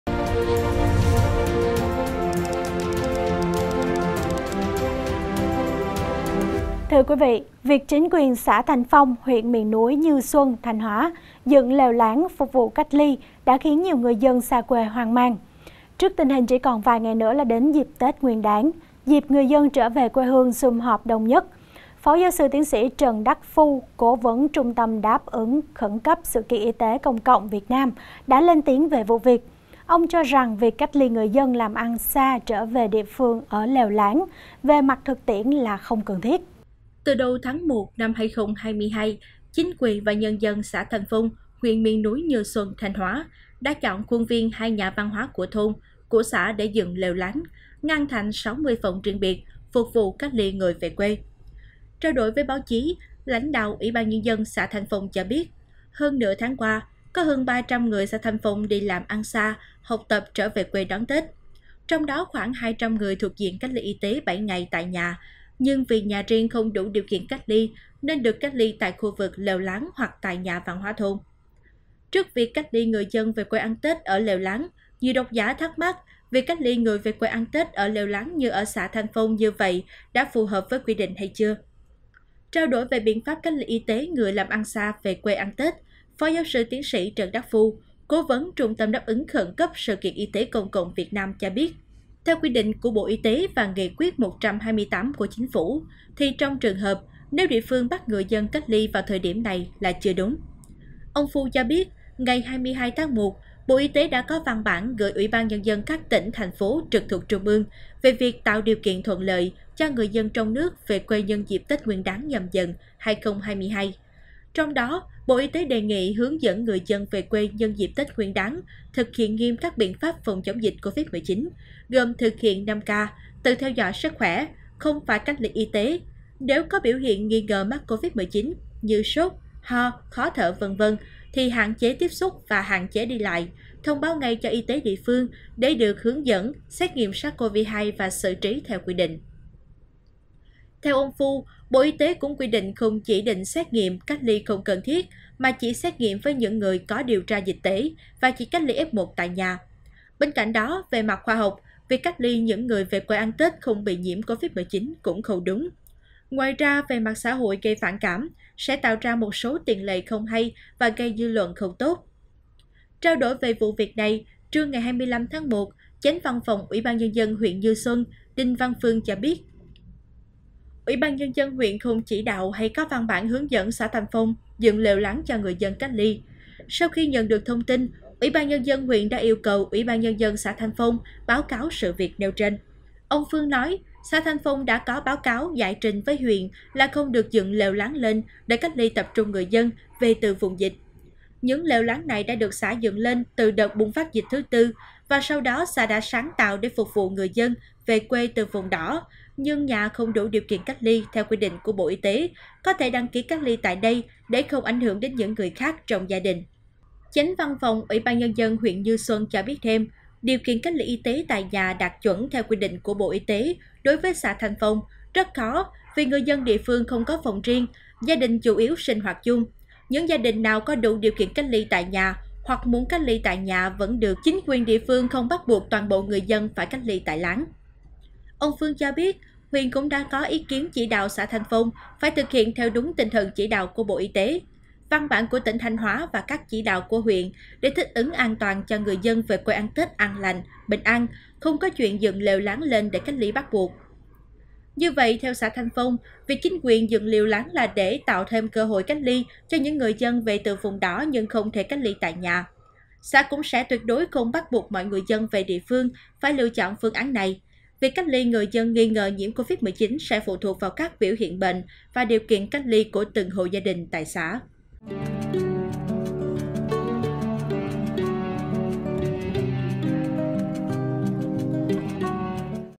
Chuyên Gia Bức Xúc Chuyện Thanh Hóa Dựng Lều Cách Ly Người Về Quê Là Không Cần Thiết | SKĐS
SKĐS | Việc chính quyền xã Thanh Phong, huyện miền núi Như Xuân (Thanh Hóa) dựng lều lán phục vụ cách ly đã khiến nhiều người dân xa quê hoang mang. Trước tình hình chỉ còn vài ngày nữa là đến Tết Nguyên đán, dịp người dân trở về quê hương sum họp đông nhất, PGS.TS. Trần Đắc Phu - Cố vấn Trung tâm Đáp ứng khẩn cấp sự kiện y tế công cộng Việt Nam đã lên tiếng về vụ việc. Ông cho rằng, việc cách ly người dân làm ăn xa trở về địa phương ở lều lán về mặt thực tiễn là không cần thiết.

Mời quý vị và các bạn xem thêm: Rợn Người Hiện Trường Nơi Nữ Sinh Đầu Độc Cha Ruột Rồi Chôn Xác, Phóng Hỏa Phi Tang